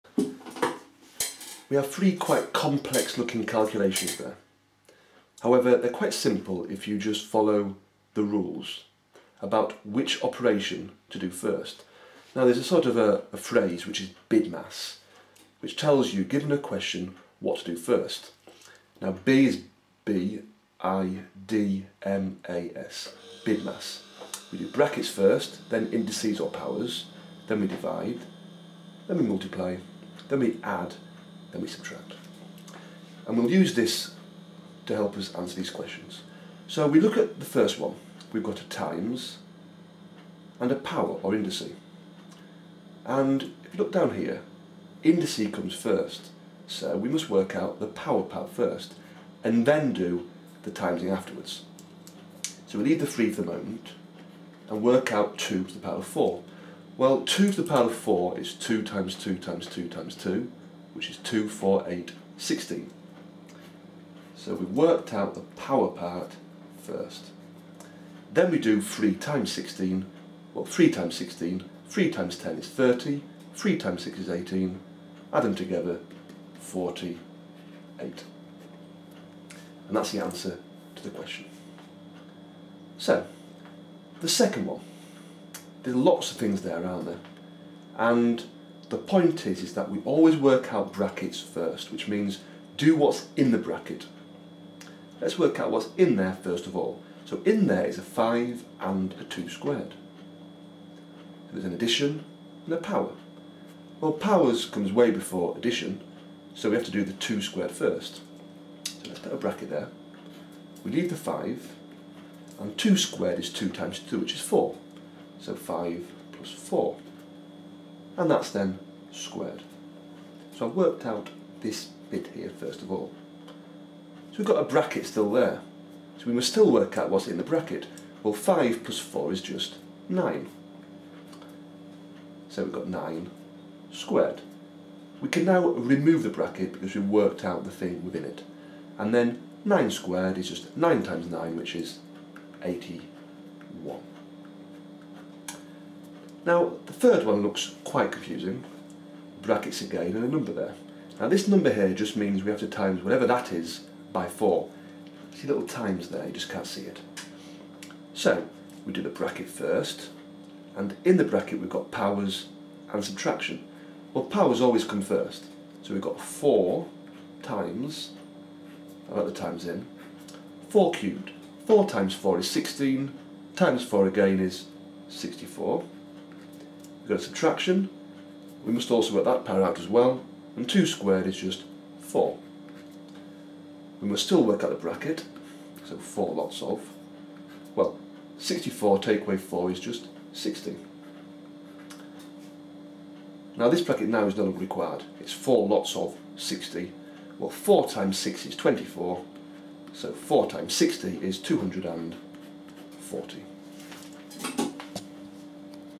Using Indices (powers) - Maths GCSE revision including BIDMAS
Powers made easy - understand powers in an instant.
This video shows how to evaluate expressions involving powers and is part of our APP videos for Key Stage three as well as providing useful GCSE Mathematics revision View my channel